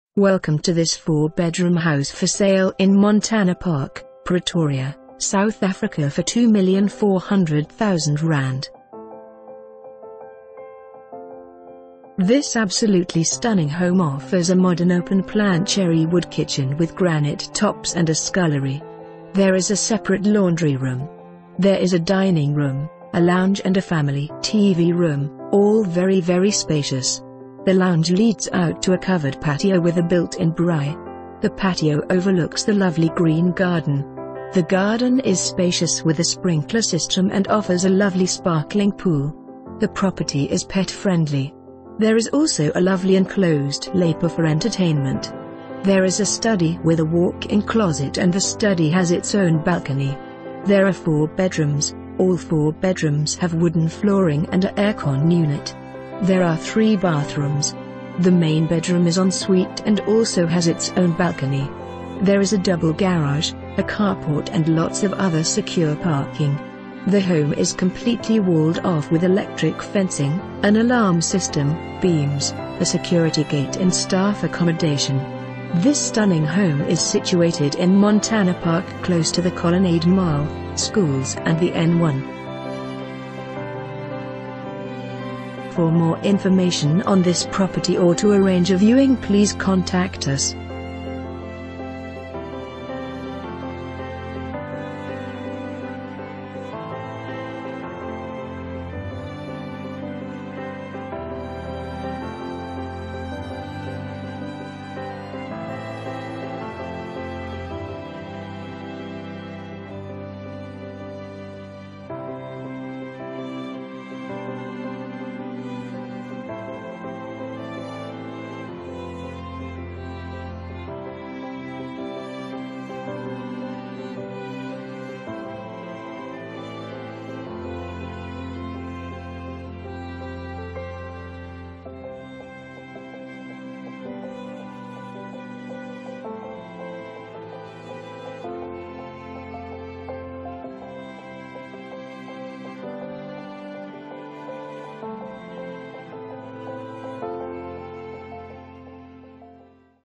4 Bedroom House For Sale in Montana Park, Pretoria, South Africa for ZAR 2,400,000...
This absolutely stunning home offers a modern open plan cherry wood kitchen with granite tops and a scullery. There is a separate laundry room.

There is a dining room, a lounge and a family/ T.V. room, all very very spacious. The lounge leads out to a covered patio with a built in braai. The patio overlooks the lovely green...